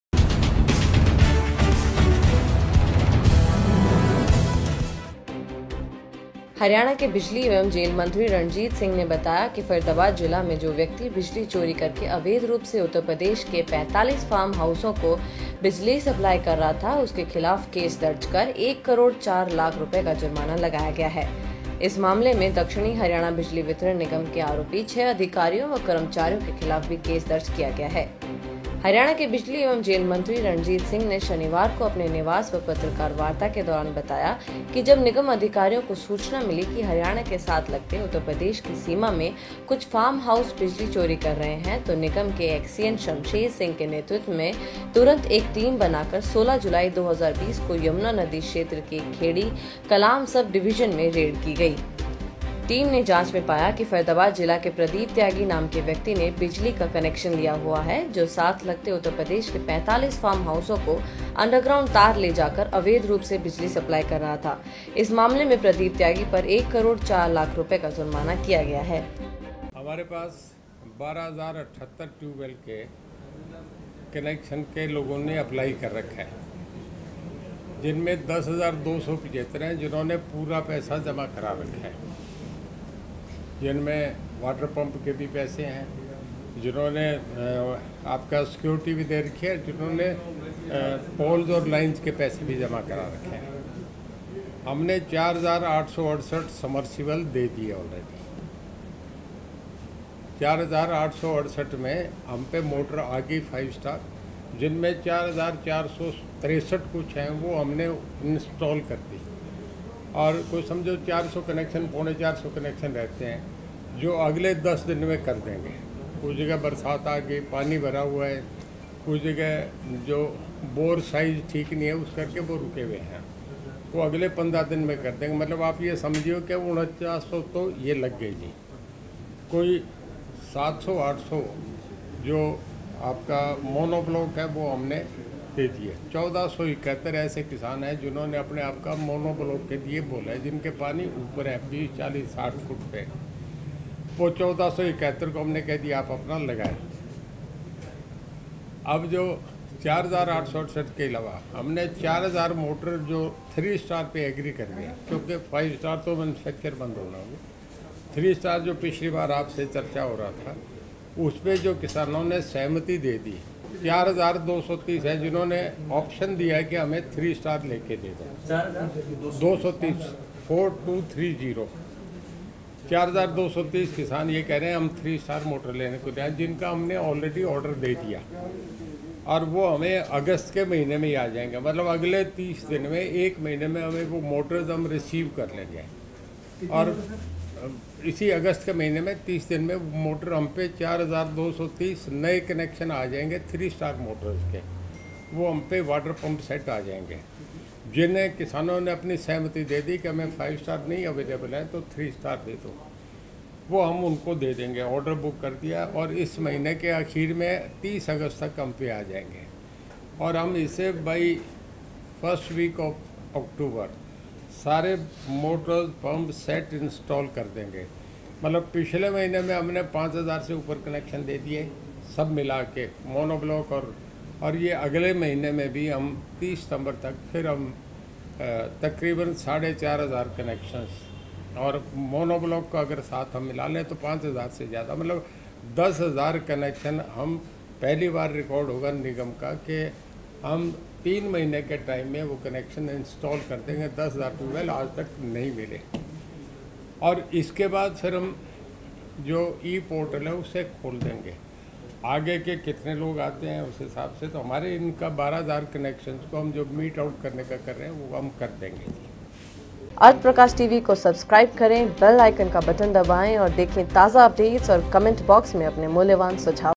मंत्री रणजीत चौटाला ने बिजली चोरी के आरोपियों पर दिखाई सख्ती, गुरुग्राम जेल के अधिकारी पर बड़ा खुलासा
हरियाणा के बिजली एवं जेल मंत्री रणजीत सिंह ने बताया कि फरीदाबाद जिला में जो व्यक्ति बिजली चोरी करके अवैध रूप से उत्तरप्रदेश के 45 फार्म-हाऊसों को बिजली सप्लाई कर रहा था उसके खिलाफ केस दर्ज कर एक करोड़ 4 लाख रूपए का जुर्माना लगाया गया है। इस मामले में दक्षिणी हरियाणा बिजली वितरण निगम के आरोपी 6 अधिकारियों व कर्मचारियों के खिलाफ भी केस दर्ज किया गया है।
हरियाणा के  बिजली एवं जेल मंत्री रणजीत सिंह ने शनिवार को अपने निवास पर पत्रकार वार्ता के दौरान बताया कि जब निगम अधिकारियों को सूचना मिली कि हरियाणा के साथ लगते उत्तरप्रदेश की सीमा में कुछ फार्म-हाऊस बिजली चोरी कर रहे हैं तो निगम के एक्सईएन शमशेर सिंह के नेतृत्व में तुरंत एक टीम बनाकर 16 जुलाई 2020 को यमुना नदी क्षेत्र के खेड़ी कलां सब-डिवीजन में रेड की गई। टीम ने जांच में पाया कि फरीदाबाद जिला के प्रदीप त्यागी नाम के व्यक्ति ने बिजली का कनैक्शन लिया हुआ है जो साथ लगते उत्तरप्रदेश के 45 फार्म-हाऊसों को अंडरग्राऊंड तार ले जाकर अवैध रूप से बिजली सप्लाई कर रहा था। इस मामले में प्रदीप त्यागी पर एक करोड़ 4 लाख रूपए का जुर्माना किया गया है।